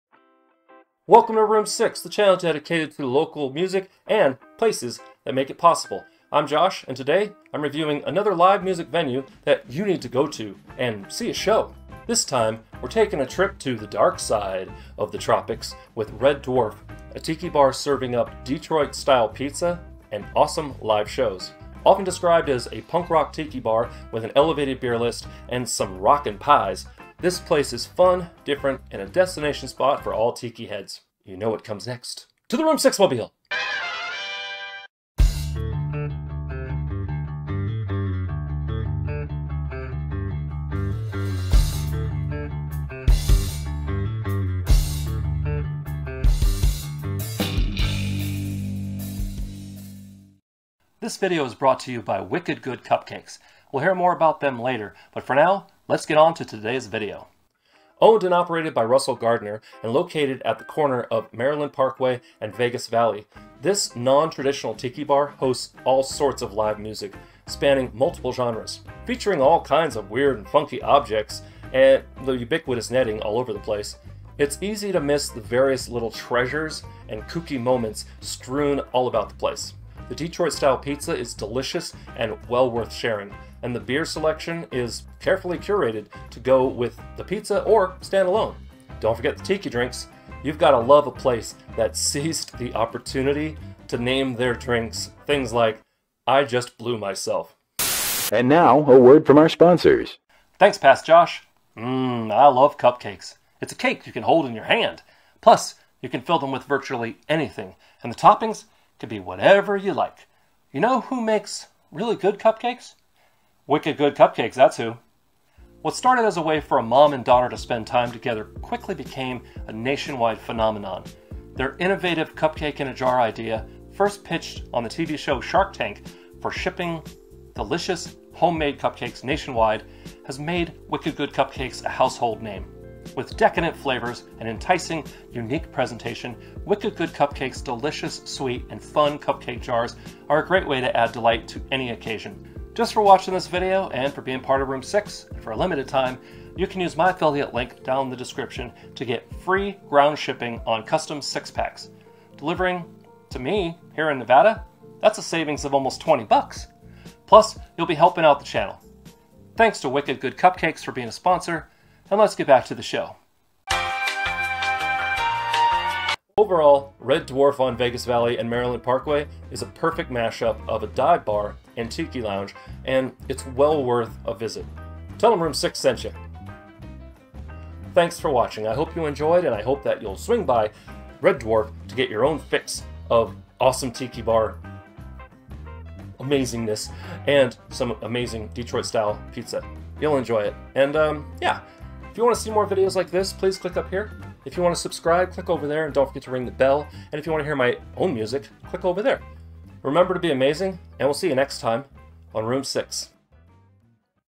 ROOM 6 REVIEWS #209 - Red Dwarf Tiki Bar! [VENUE REVIEW]
Today I'm reviewing RED DWARF, an awesome local tiki bar serviing up Detroit-style pizza and live music. Enjoy! ;-D